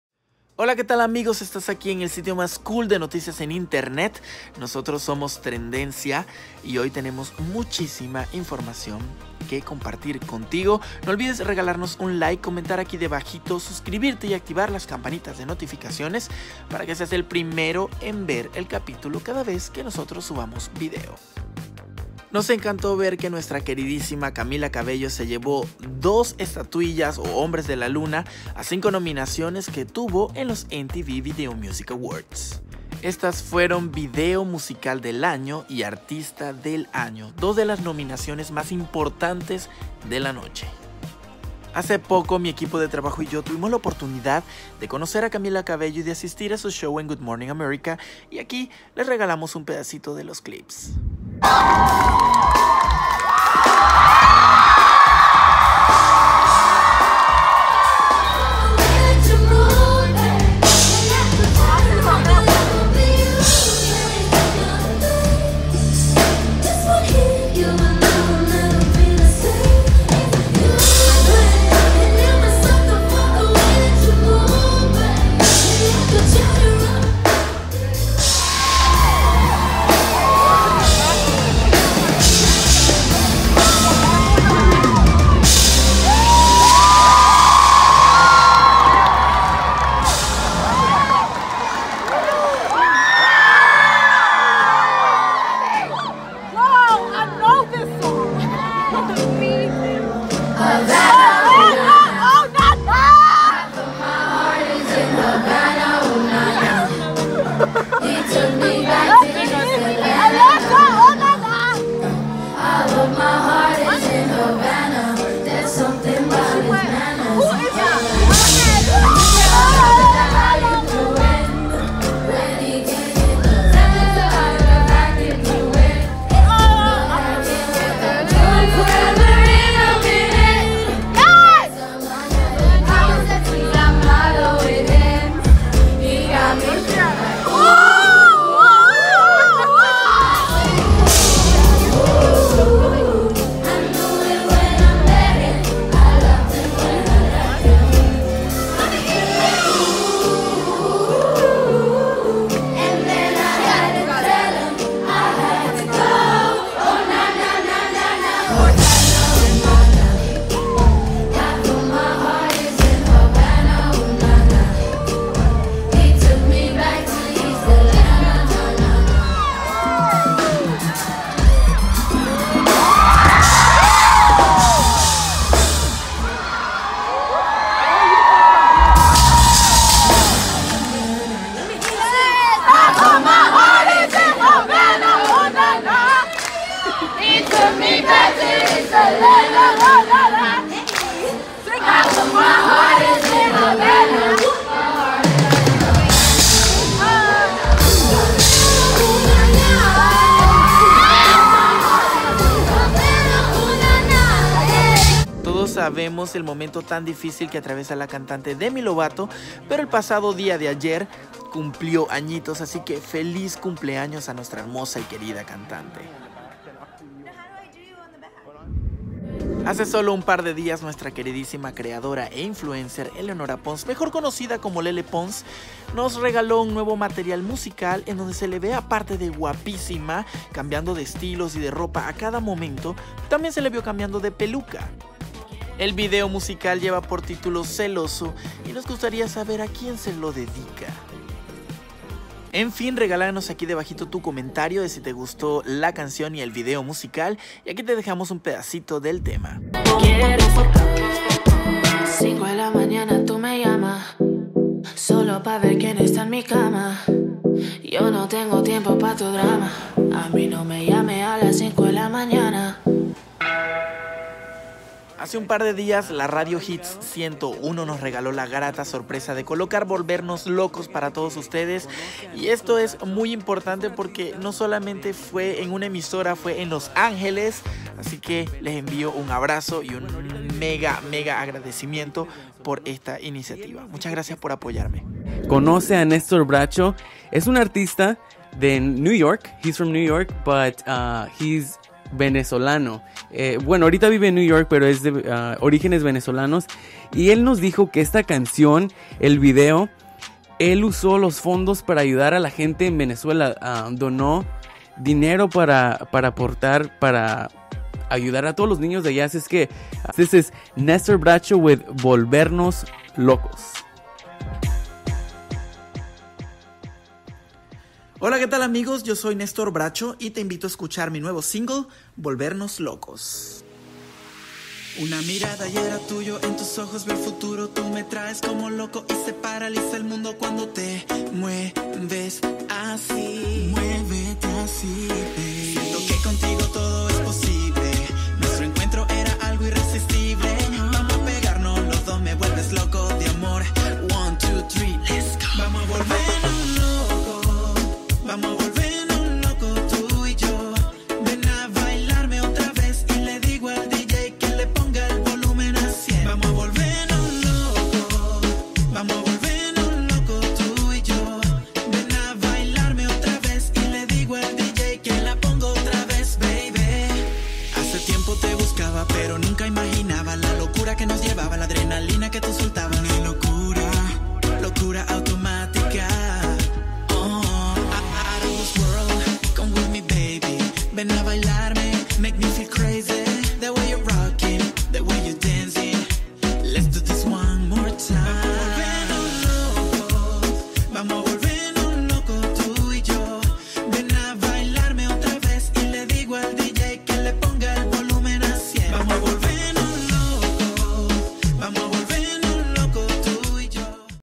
Camila Cabello arrasa en VMA 2018 y Lele Pons Estrena Vídeo Musical
Todo este y más aquí en en somos trendencia
Cual notita te gusto más